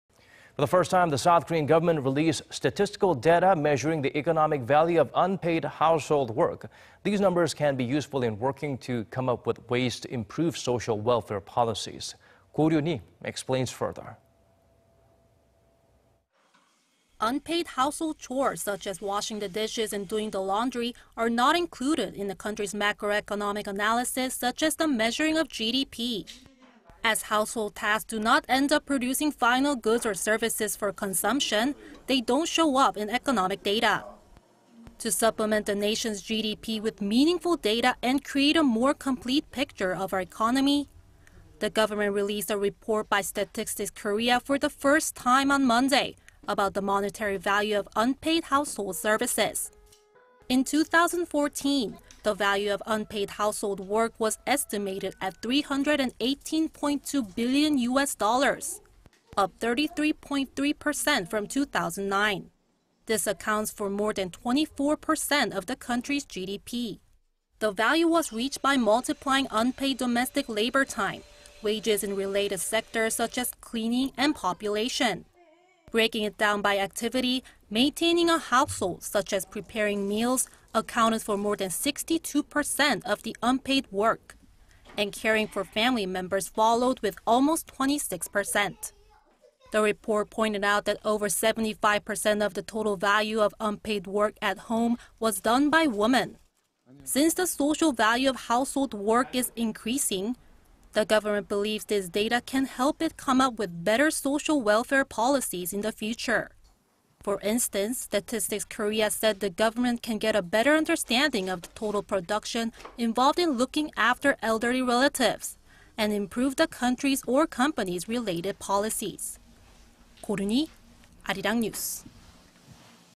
S. Korea's unpaid household service work valued around US$ 318.2 bil. in 2014: gov't data
자녀돌보기 등 가사노동가치 연간 361조원…GDP대비 24.3%

  For the first time, the South Korean government released statistical data measuring the economic value of unpaid household work.
These numbers can be useful in working to come up with ways to improve social welfare policies.
Ko Roon-hee explains further.
 Unpaid household chores such as washing the dishes and doing the laundry are not included in a country's macroeconomic analysis, such as the measuring of GDP.

As household tasks do not end up producing final goods or services for consumption, they don't show up in economic data.

 To supplement the nation's GDP with meaningful data and create a more complete picture of our economy, the government released a report by Statistics Korea for the first time on Monday about the monetary value of unpaid household services.

 In 2014, the value of unpaid household work was estimated at 318-point-2 billion U.S. dollars…up 33-point-3 percent from 2009. This accounts for more than 24-percent of the country's GDP.

The value was reached by multiplying unpaid domestic labor time, wages in related sectors such as cleaning, and population.

 Breaking it down by activity, maintaining a household, such as preparing meals, accounted for more than 62-percent of the unpaid work, and caring for family members followed with almost 26-percent.

The report pointed out that over 75-percent of the total value of unpaid work at home was done by women.

 Since the social value of household work is increasing, the government believes this data can help it come up with better social welfare policies in the future.
 For instance, Statistics Korea said the government can get a better understanding of the total production involved in looking after elderly relatives…and improve the country's or companies' related policies.
Ko Roon-hee, Arirang News.